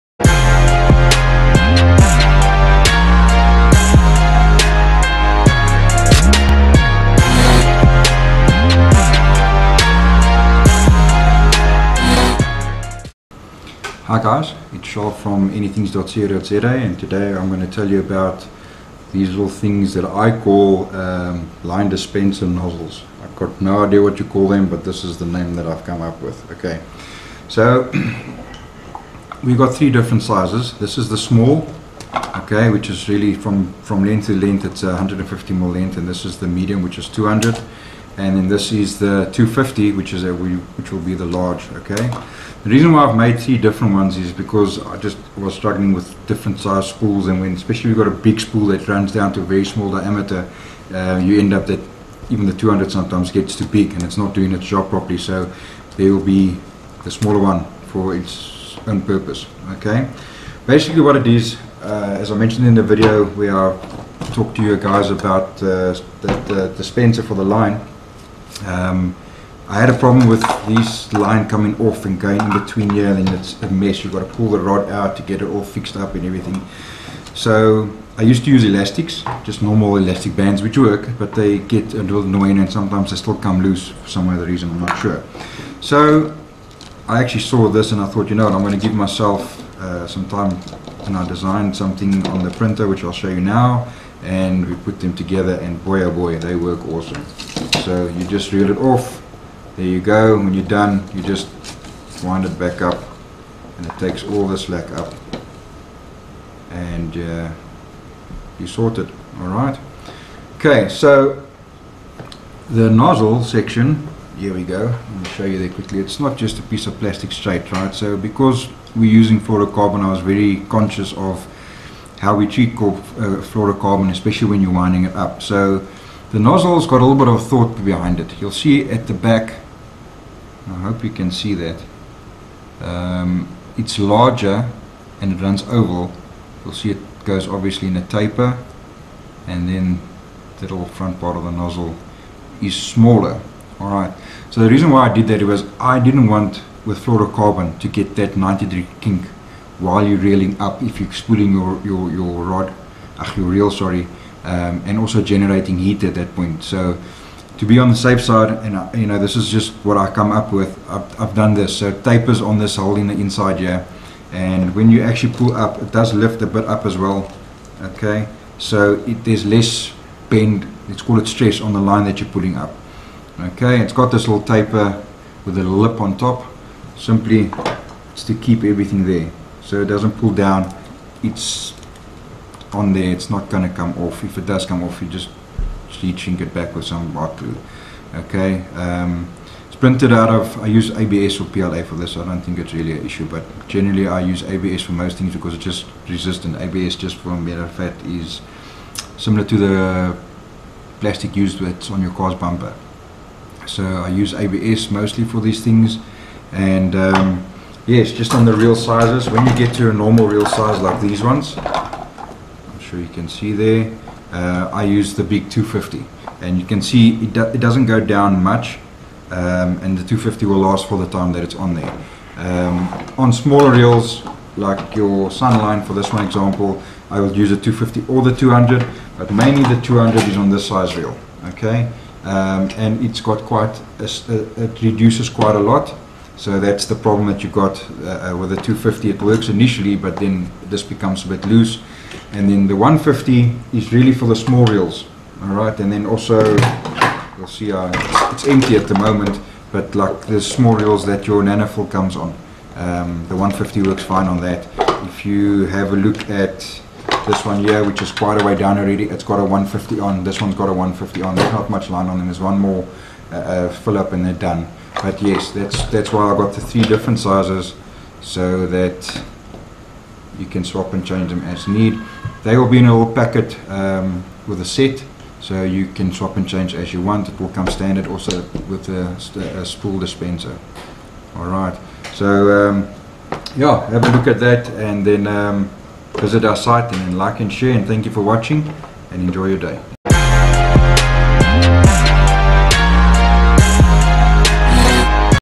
Line Dispenser Nozzles
The nozzles come in 3 sizes S, M and L. Here I explain how and when to use them on your spools. Works Great with the Line Spool Dispenser.